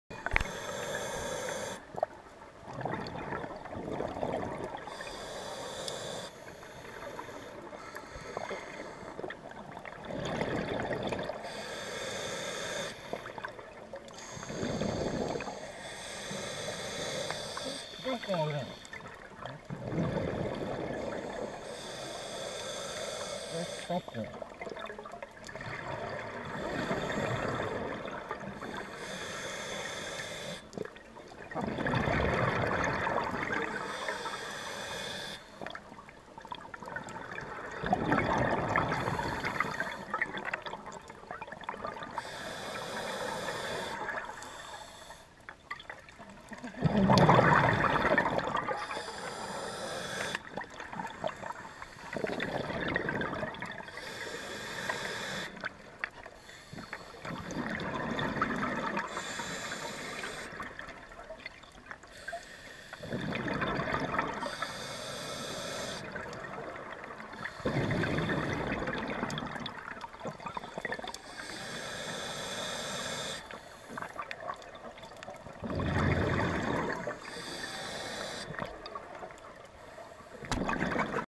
Vandenberg conch+bubbles
March 2014, Vandenberg wreck off Key West. Conch sucked into the deck, Vika with Chris, her instructor from Lost Reef Adventures. Lots of bubbles.